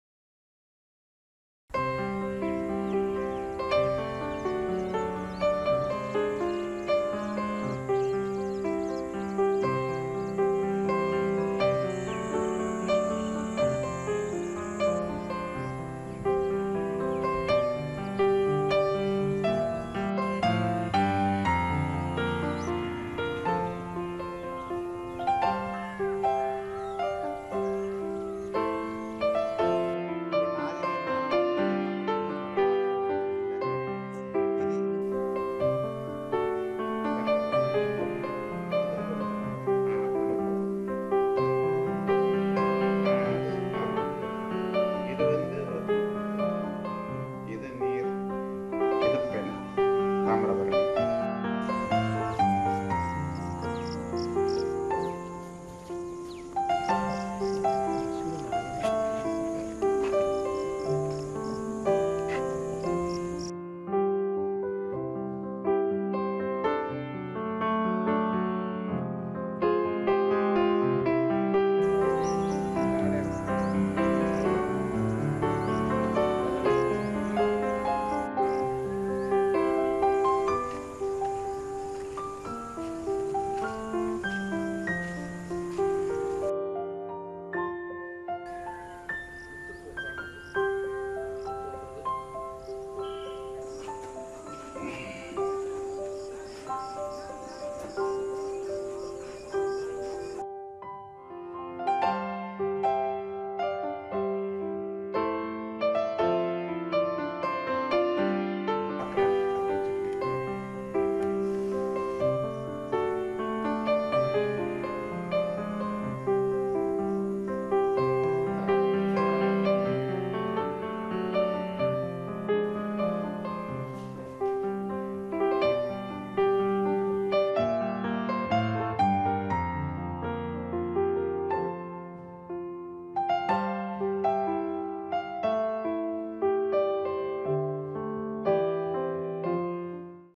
Guru Vanam: Artist Chandru’s park of statues
Since 2019, sculptures have been filling up Guru Vanam, an open museum that stands for modern sculpting. The open museum flaunts 54 statues made of clay and fibre. Guru Vanam has been curated by G Chandrasekaran, also called Chandru.